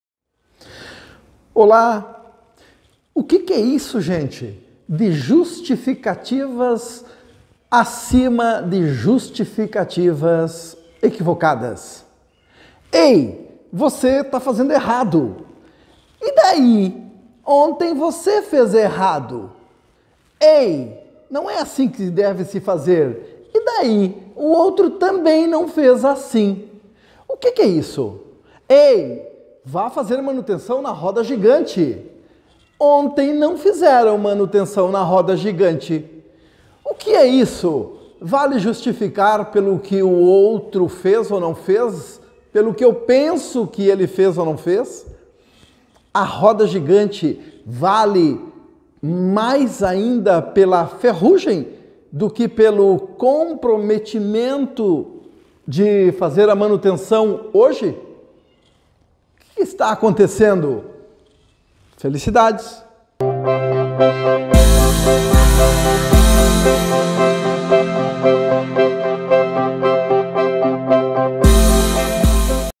Comportamento | Justificativas equivocadas
Você está fazendo errado!
Ontem foi você que fez errado!

As justificativas equivocadas, são o tema do Quadro Comportamento desta semana, com Antonio Gomes da Rosa.